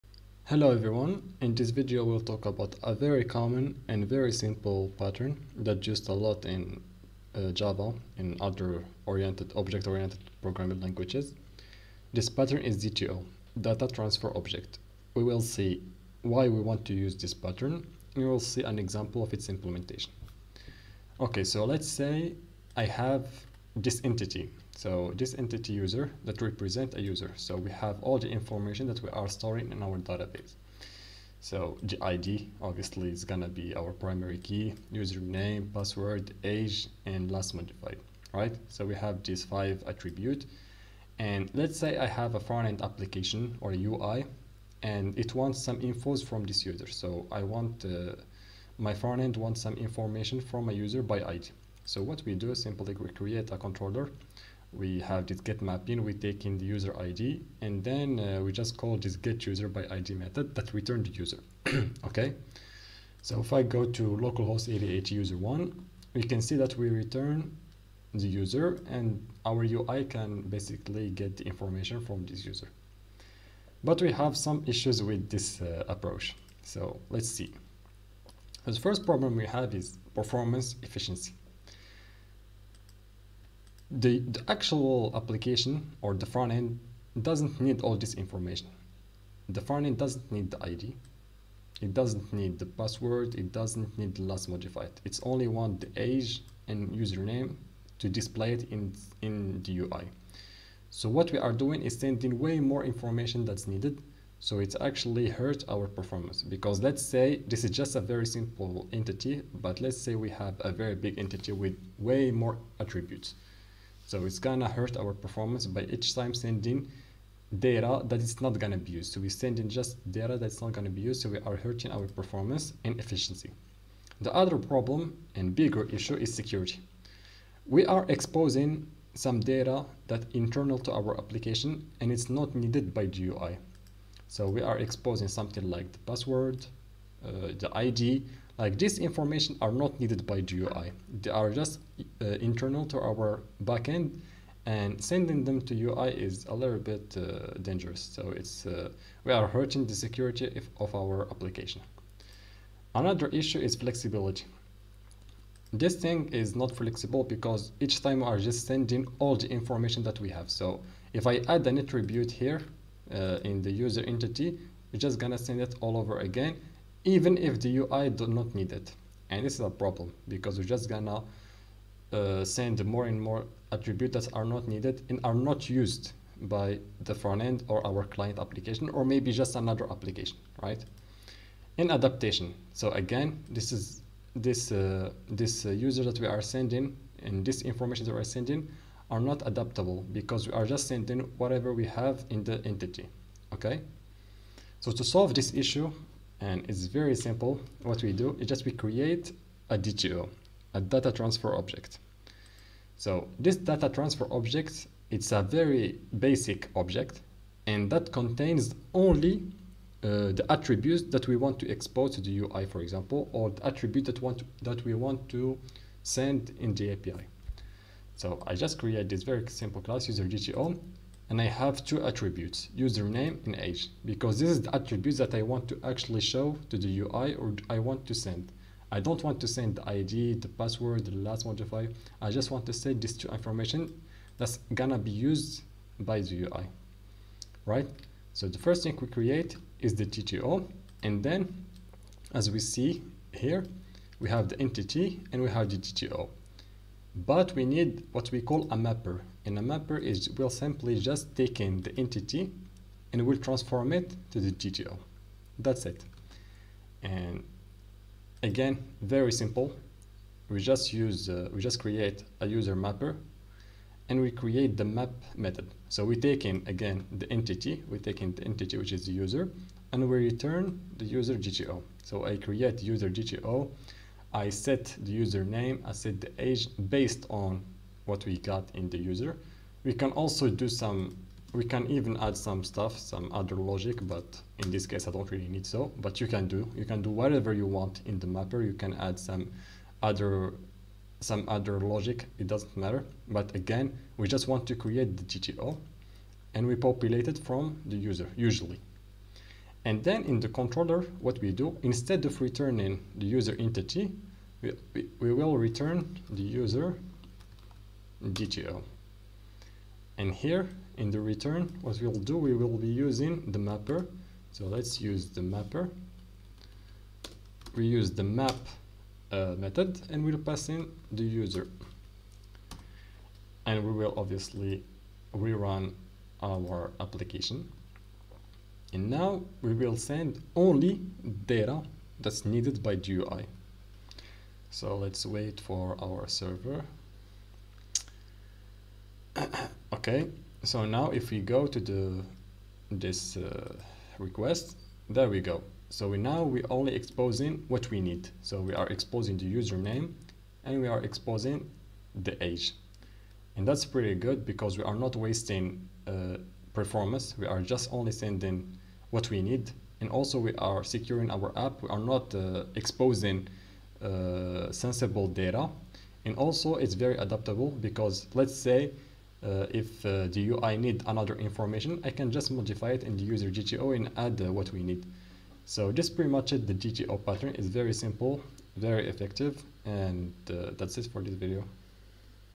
What is the DTO pattern and why we use it. (Java)
- in this video we go over the DTO pattern where we see a simple use case of this pattern and why we use it. :)